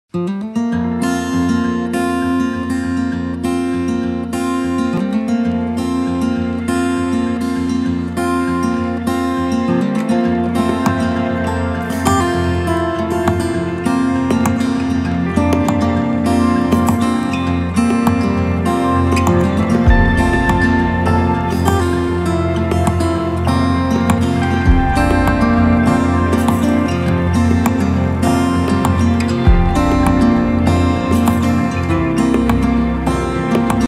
في اليوم 19 من رمضان..أمطار متفرقة ورياح قوية "اليوم الأحد"
تتوقع المديرية العامة للأرصاد الجوية، بالنسبة ليوم الأحد ، ظهور سحب ركامية و غير مستقرة شيئا ما فوق مرتفعات الأطلسين الكبير و المتوسط مرفوقة بقطرات مطرية متفرقة و بروز رعد محلي.